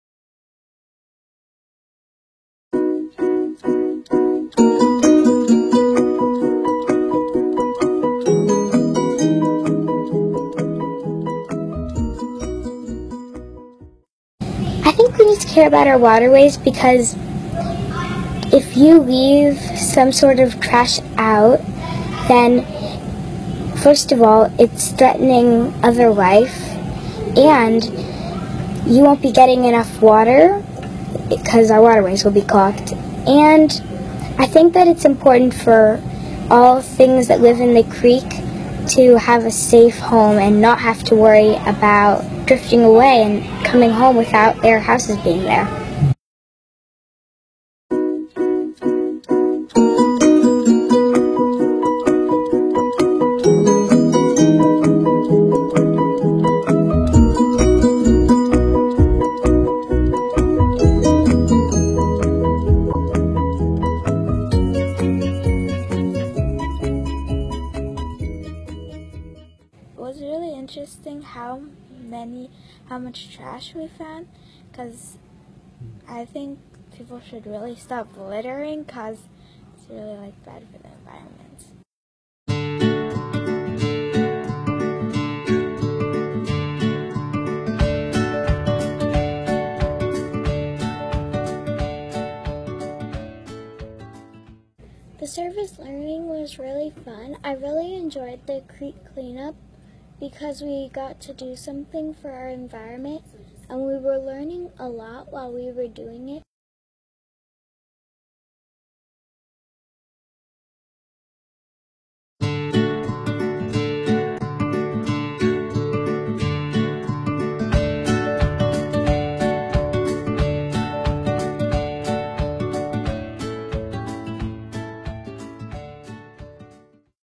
Youth Eco Steward YES Program
Video created by students and faculty at Mulberry School following one of their field days.